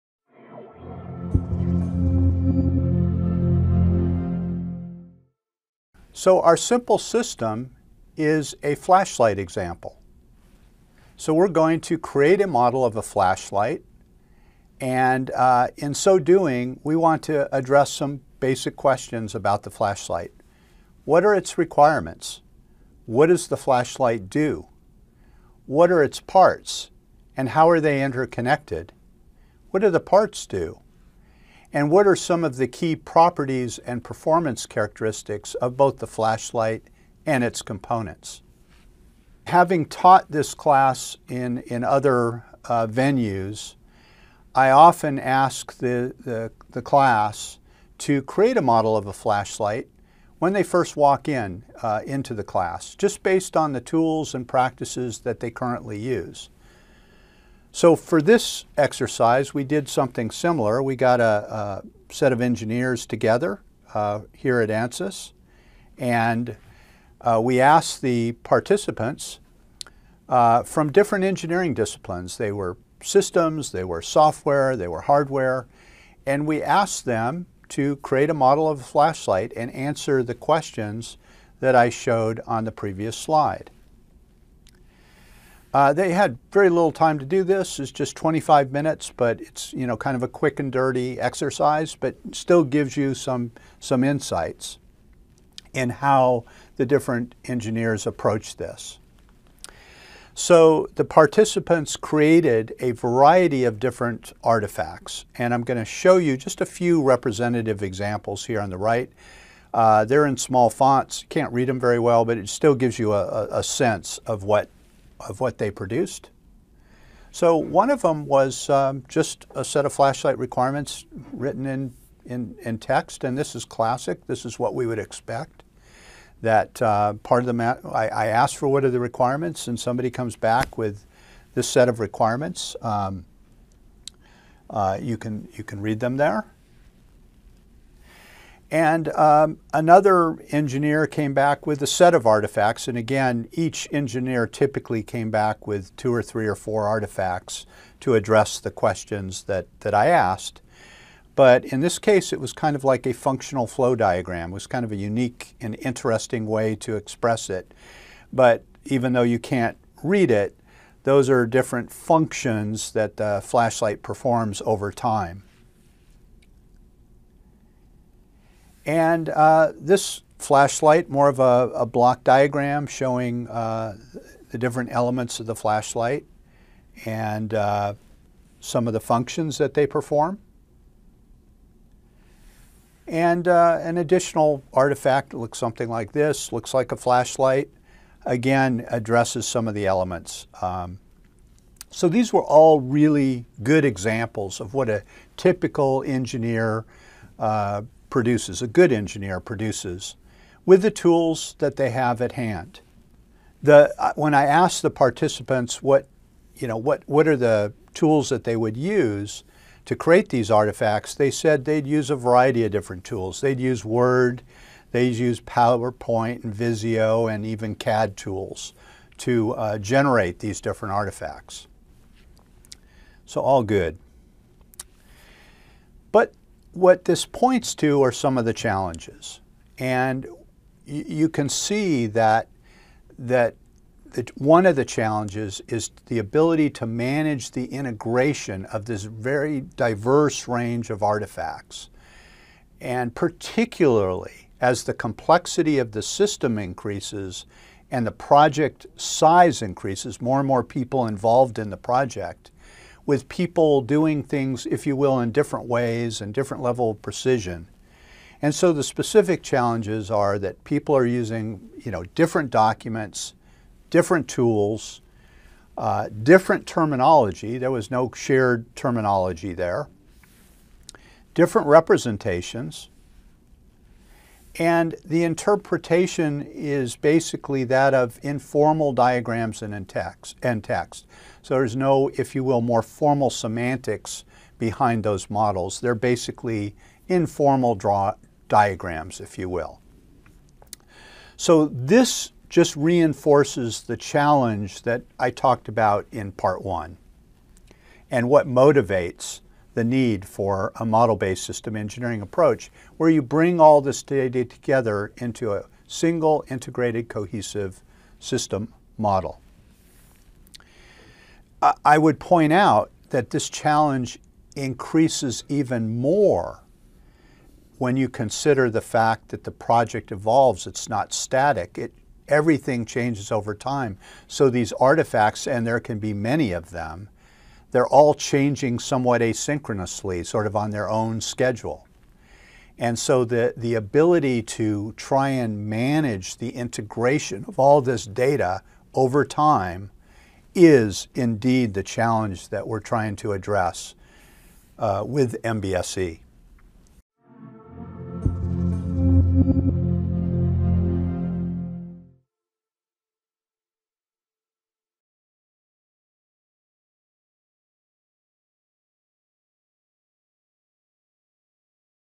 Modeling a System with SysML using Flashlight Example — Lesson 1
In this lesson of the Ansys Course, explore the process of creating a flashlight model in this lesson. Understand its requirements, parts, and performance characteristics. Learn about the challenges engineers face in integrating diverse artifacts, especially in complex systems and large projects. Discover the importance of a model-based system engineering approach for a cohesive, integrated system model. This lesson provides valuable insights for anyone interested in system modeling and engineering.

Chapters —

00:05 – Flashlight modeling
00:38 – Create model/artifacts of flashlight
03:44 – Challenges in artifacts management